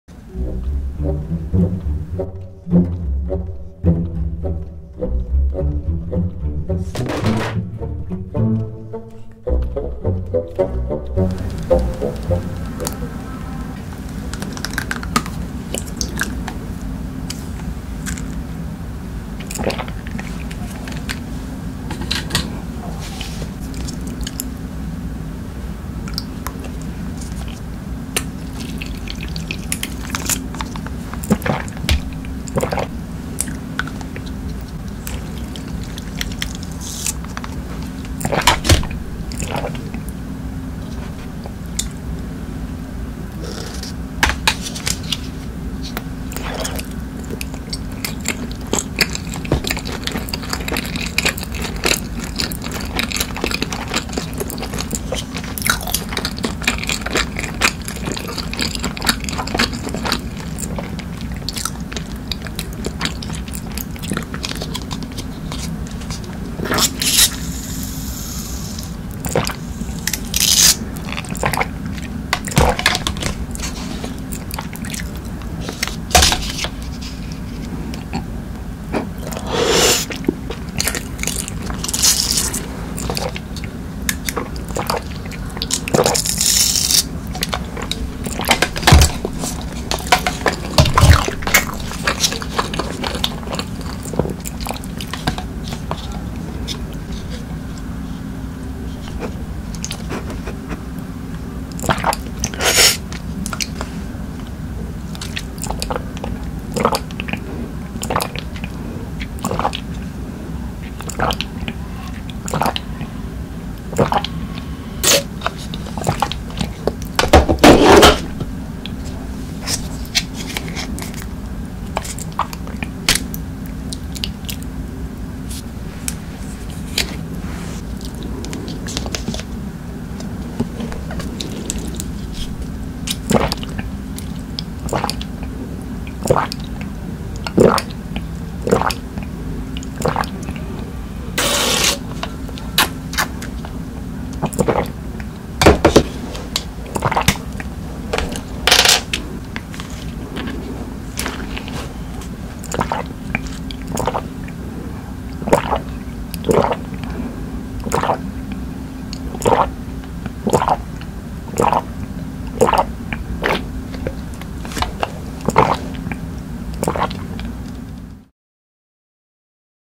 TIKTOK SYRINGE DRINK ASMR TIKTOK SYRINGE MUKBANG 틱톡 주사기 음료수 먹방 ASMR
Hi guys !
Today I prepared Tiktok syringe juice !
xx